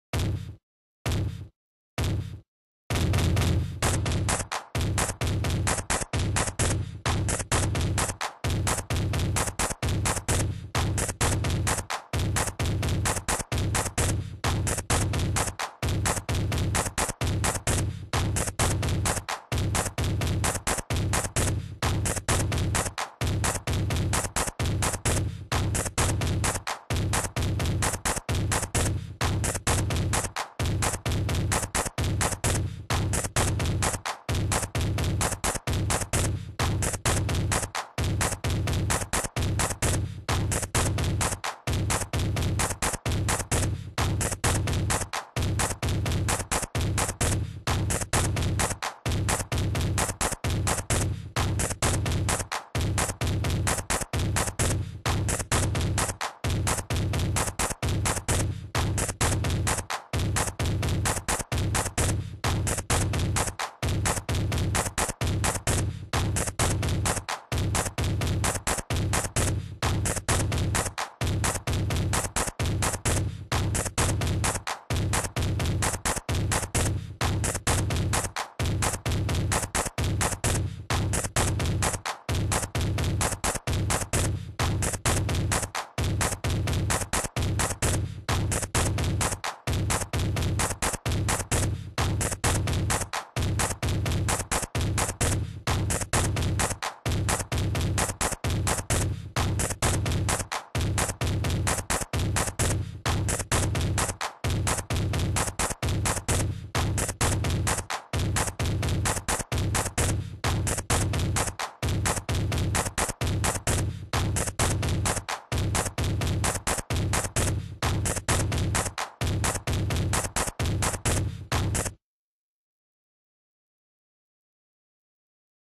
YBS BEATS
DIS IS JUST A BEAT I MADE USING REASON 3.0 NOTHING SPECIAL THOUGH JUST THOUGHT I'LL UPLOAD IT HOPEFULLY MY GROUP WILL BE USING IT 4 A TRACK.
I NEED SUM ONE TO ADD A PIANO PART 4 ME PLZ AND WE'LL SEE WUT HAPPENS
14 YEARS OLD PRODUCER IN THE MAKEING